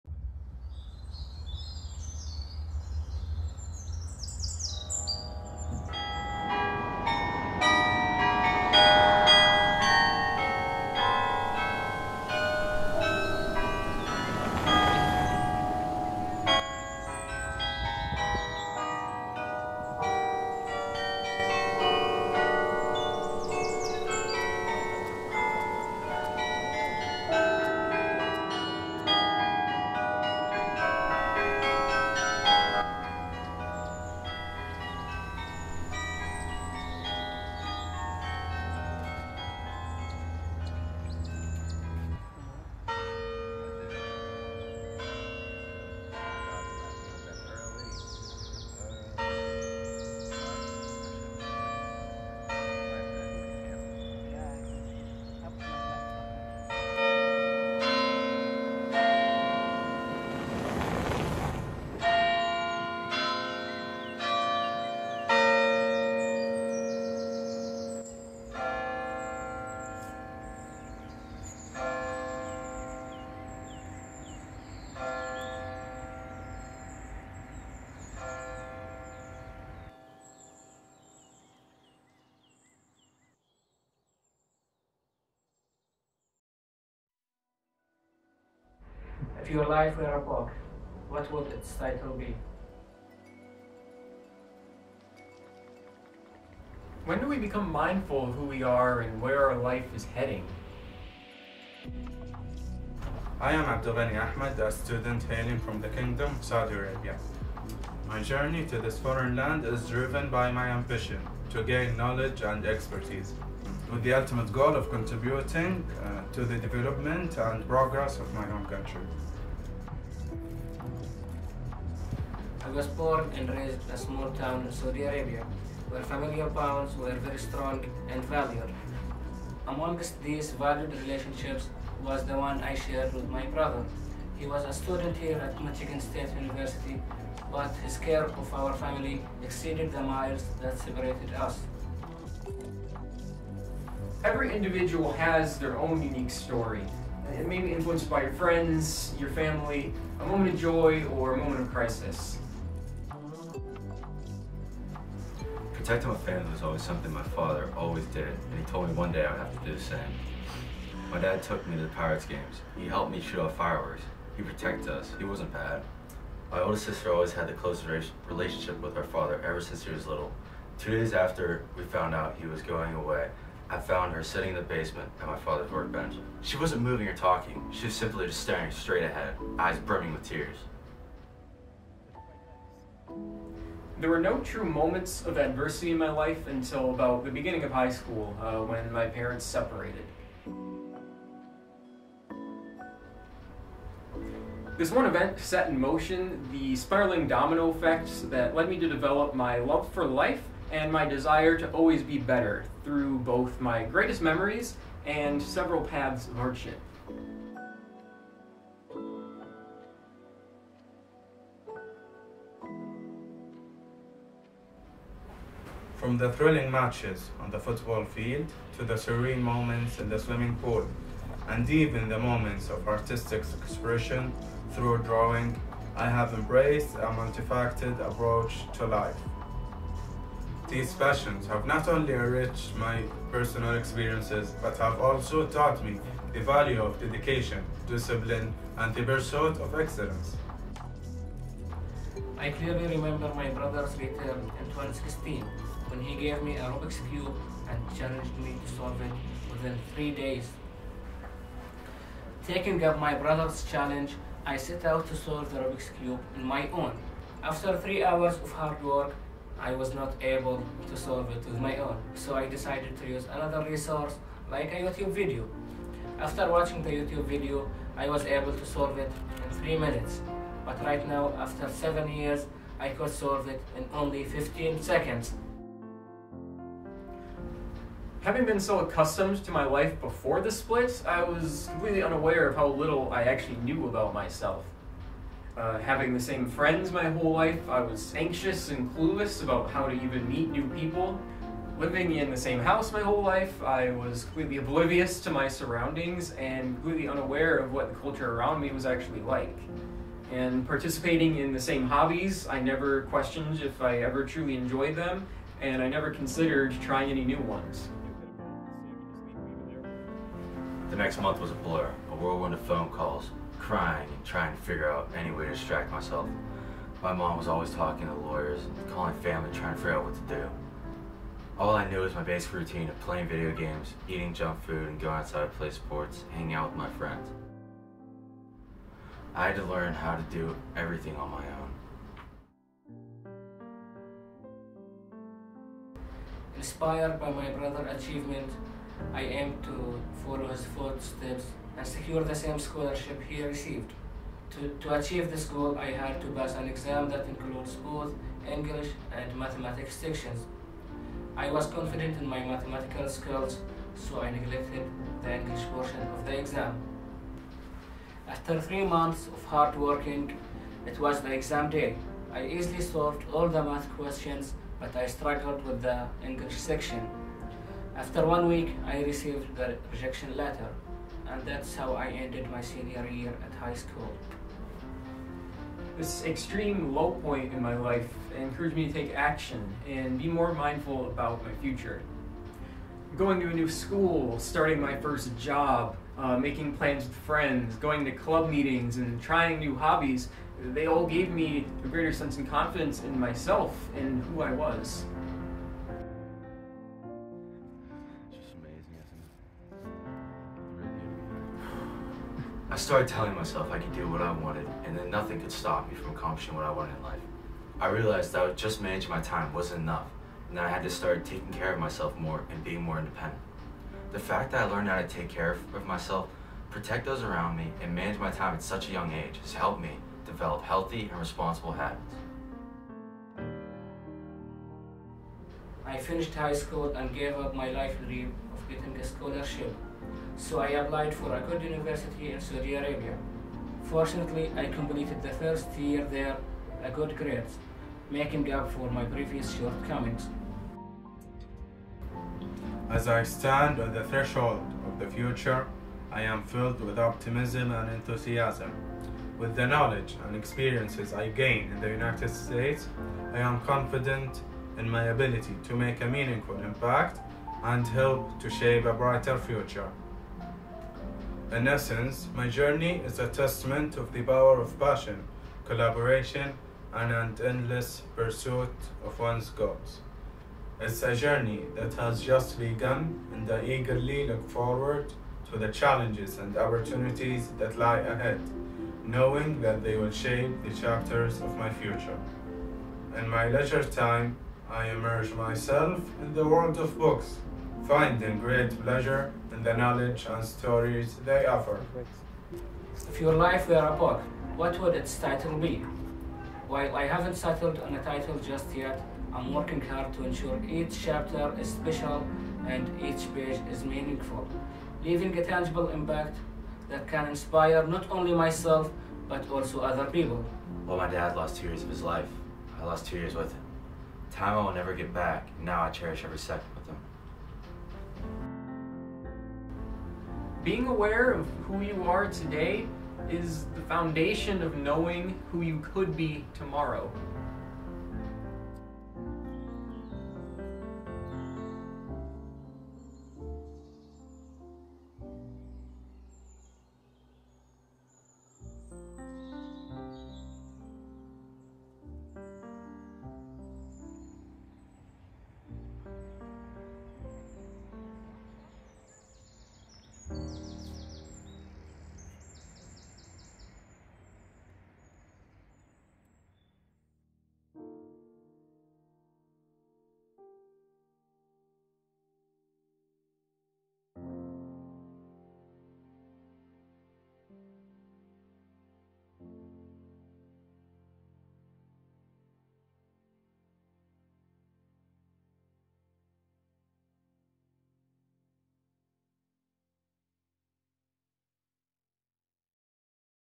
Our Journey
A college project practicing my video editing skills. This short film is a joint narrative of four individual essays narrated by their respective authors, highlighting the similarities in our journeys to college, despite our differences.